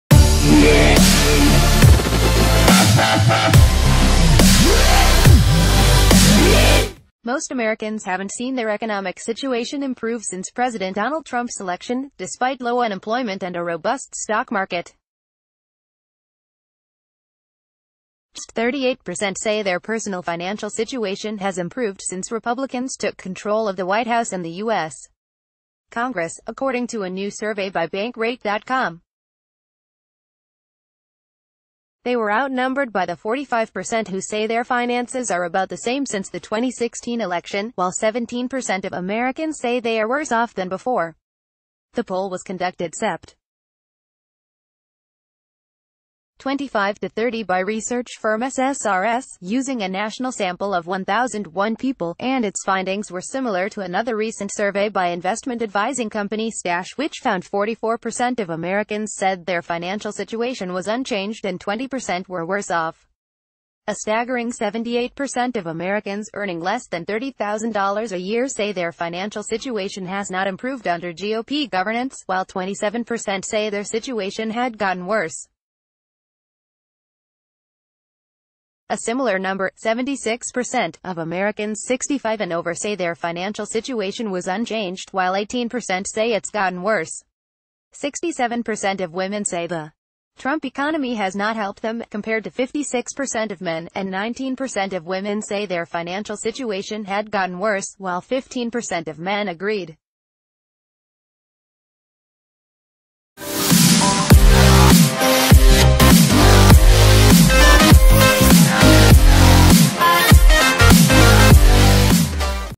Daily News - The majority of Americans say that Trump economy has left them behind - or doing thi...
Daily News - The majority of Americans say that Trump economy has left them behind - or doing things worse

Most Americans haven’t seen their economic situation improve since President Donald Trump’s election — despite low unemployment and a robust stock market. Just 38 percent say their personal financial situation has improved since Republicans took control of the White House and the U.S. Congress, according to a new survey by Bankrate.com. They were outnumbered by the 45 percent who say their finances are about the same since the 2016 election, while 17 percent of Americans say they are worse off t...